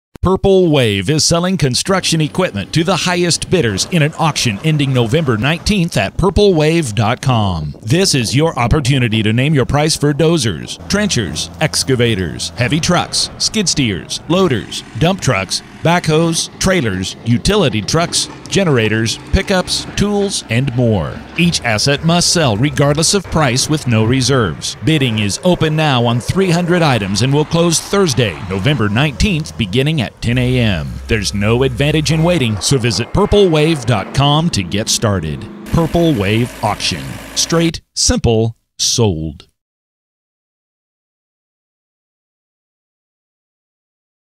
Construction equipment auction | November 19, 2015 | Purple Wave
300 items in this absolute auction will begin to close at 10 a.m. CST on Thursday, November 19, 2015.

Purple Wave is selling construction equipment to the highest bidders in an auction ending November 19 at purplewave.com. This is your opportunity to name your price for dozers, trenchers, excavators, heavy trucks, skid steers, loaders, dump trucks, backhoes, trailers, utility trucks, generators, pickups, tools and more. Each asset must sell regardless of price with no reserves. Bidding is open now on 300 items and will close Thursday, November 19, beginning at 10 a.m. central. There's no advantage in waiting, so visit purplewave.com to get started. Purple Wave Auction. Straight. Simple. Sold.